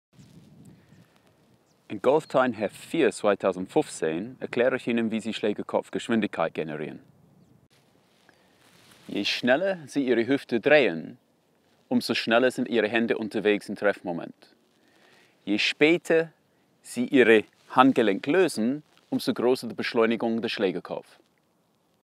Golftraining mit Jonathan Taylor (Quick-Tipp): Individuelles Timing II
In diesem kurzen Clip gibt GOLF TIME-Autor Jonathan Taylor von der The Move Academy Tipps für mehr Schlägerkopfgeschwindigkeit und somit mehr Weite. Die dazugehörige, ausführliche Trainingsstrecke lesen Sie in GOLF TIME 4/2015.